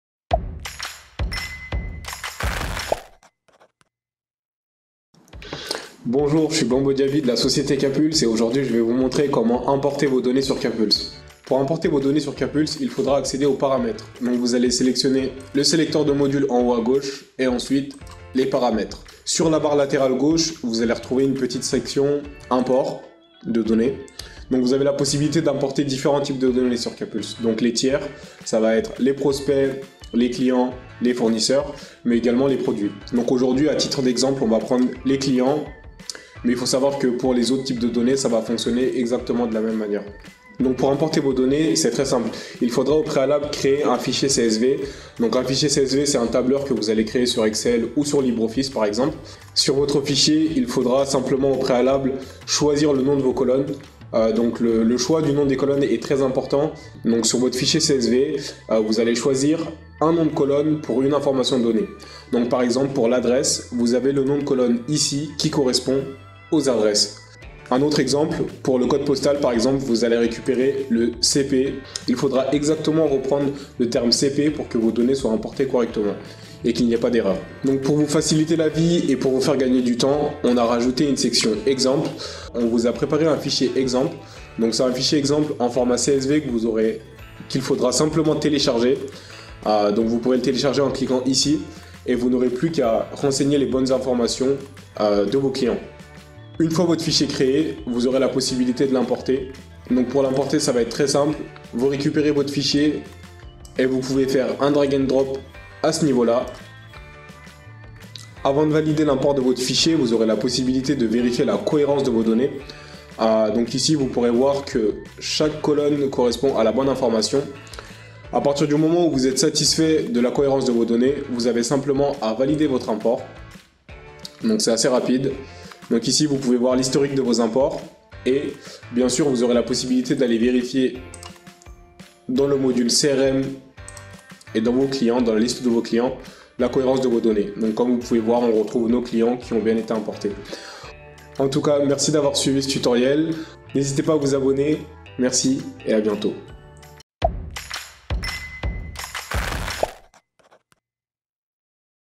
Importer vos données - Le Guide
Découvrez comment importer vos données client, prospect, fournisseurs ou produit dans KPulse depuis un fichier CSV !